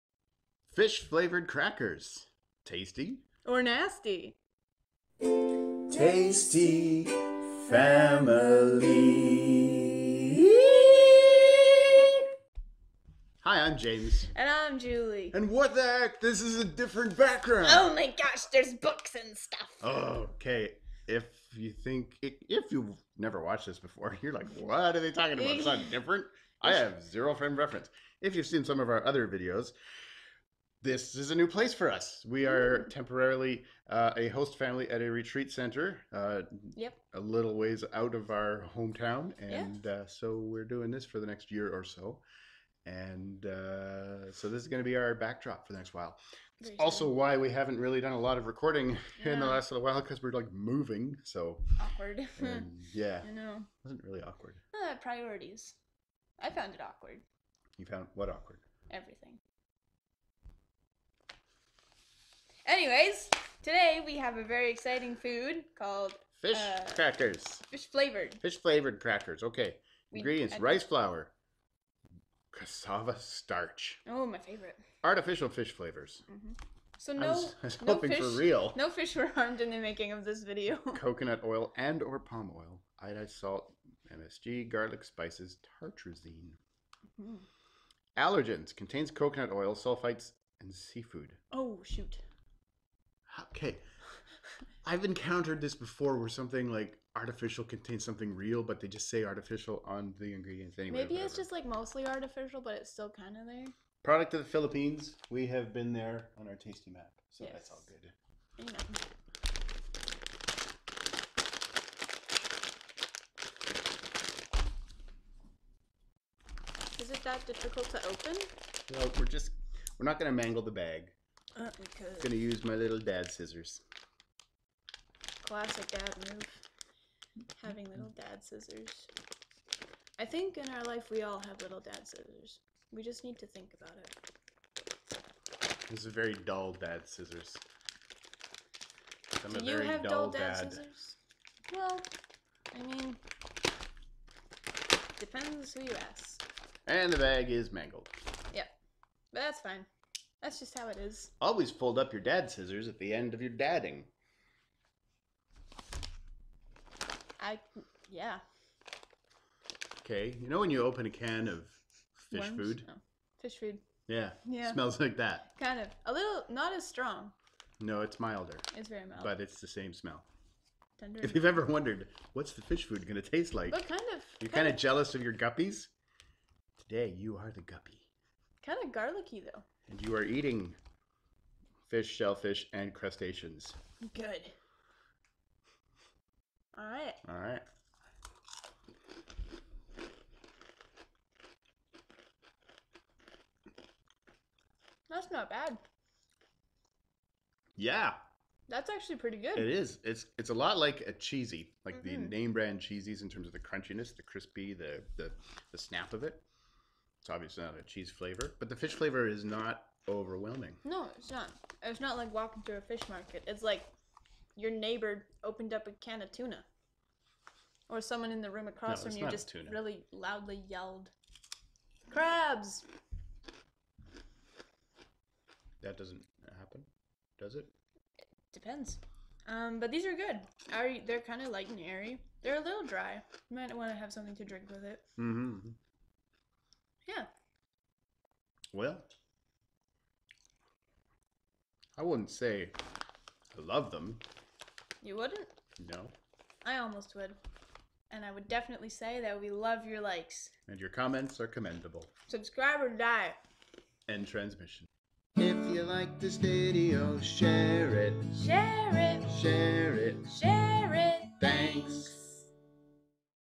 Trying Fish-Flavoured Crackers
These crispy cheezy-like crackers were surprisingly palatable.

Tasty Family episode #301